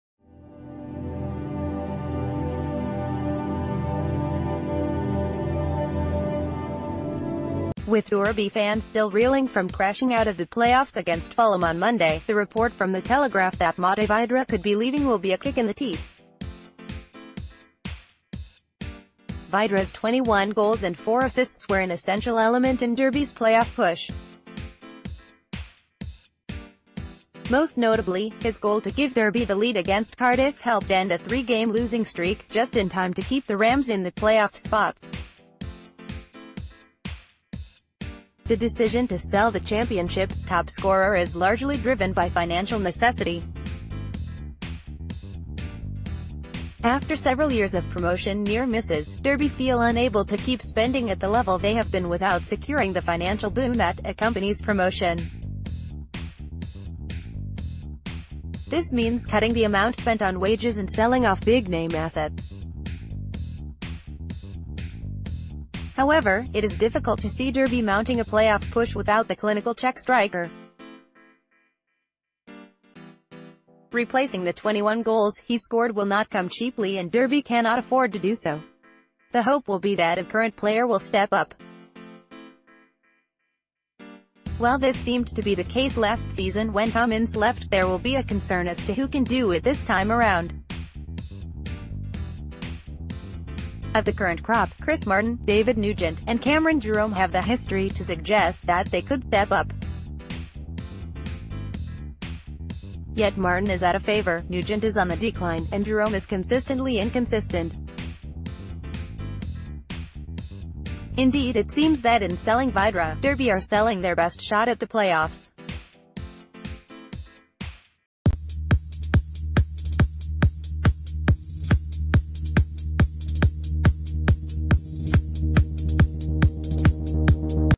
Breaking News | Are Derby selling their play-off hopes for next season?
With Derby fans still reeling from crashing out of the play-offs against Fulham on Monday, the report from the Telegraph that Matej Vydra could be leaving will be a kick in the teeth. Vydra’s 21 goals and four assists were an essential element in Derby’s play-off push. Most notably, his goal to give Derby the lead against Cardiff helped end a three game losing streak, just in time to keep the Rams in the play-off spots. The decision to sell the Championship’s top scorer is largely driven by fina...